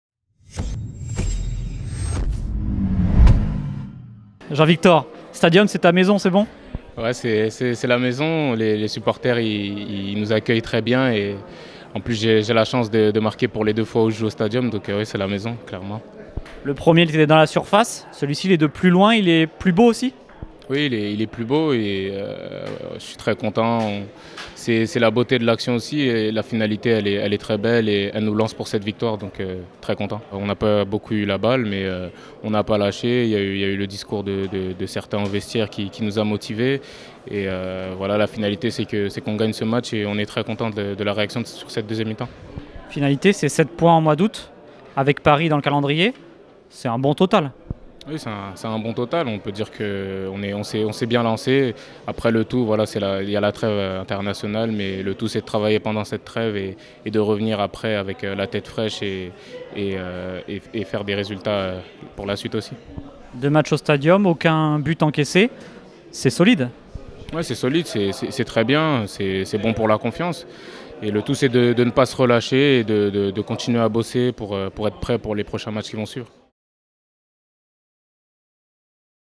"L'action de mon but est très belle", l'interview de Jean-Victor Makengo après TFC/Amiens
Auteur de l'ouverture du score des Violets face à Amiens d'une frappe lointaine - son 2ème but en deux matchs au Stadium -, le milieu de terrain Jean-Victor Makengo a répondu aux questions de TFC TV.